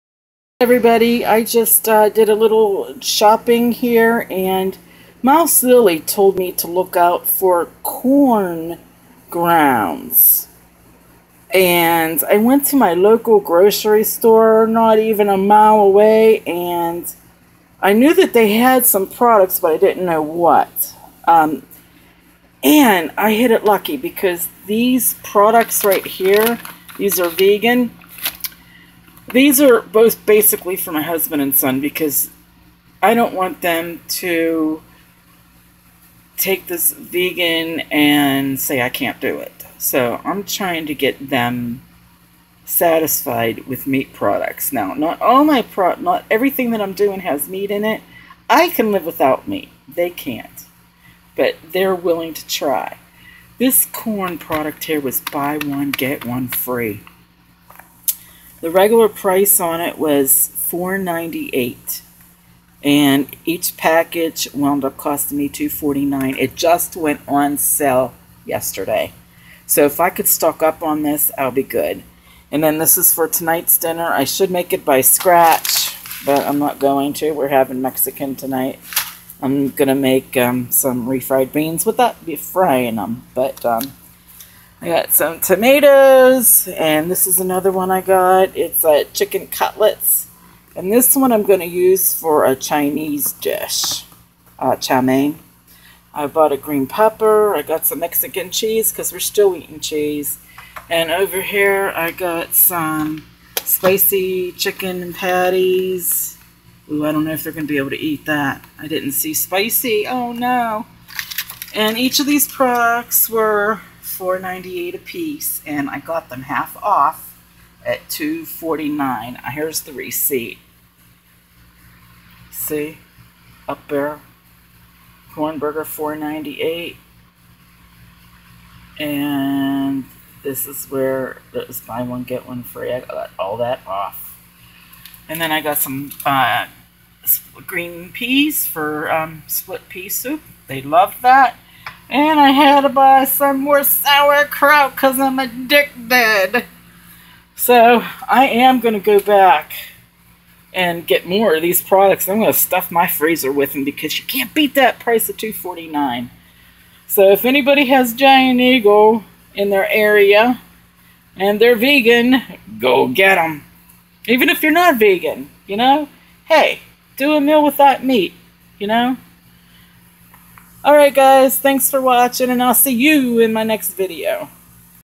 SMALL VEGAN GROCERY HAUL**SAVED $16.00
First time I hit the jackpot on the weekly sales. I was there at the right time. Buy one get one free! going back to get more at another store with more variety that carries 9 items of Quorn.
Thank you Mouselily for all the information you gave me.  If it wasn't for you, I wouldn't have looked for then and then I would have never of gotten this great deal.  thank you so much!!!